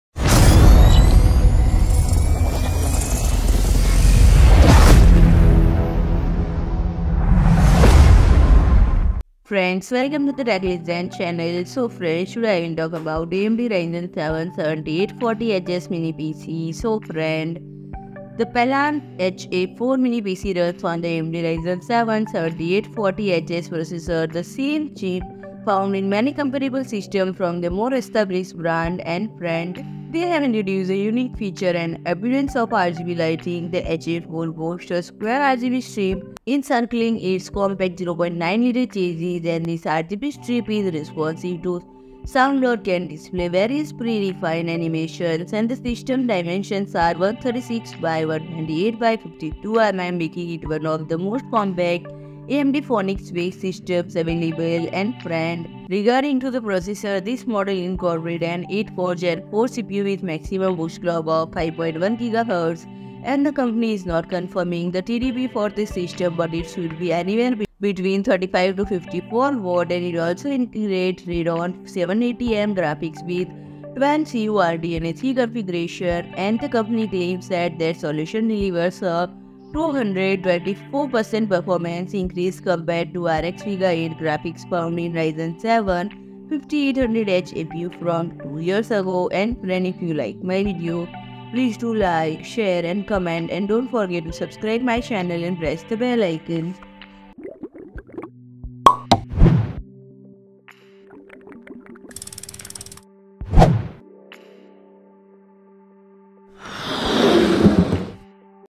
Peland AMD Ryzen 7 7840HS Mini-PC Launched With RGB Lighting - Explained All Spec, Features & More
PRODUCT BRAND STORE AMAZON

PELAND HA-4 mini-PC runs on the AMD Ryzen 7 7840HS processor, the same chip found in many comparable systems from more established brands.